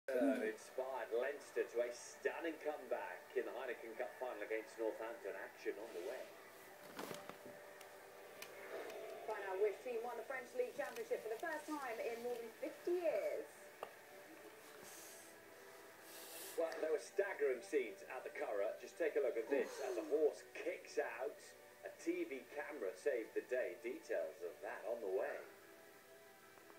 Cameraman gets kicked off horse on At The Races
Atr cameraman gets kicked off a horse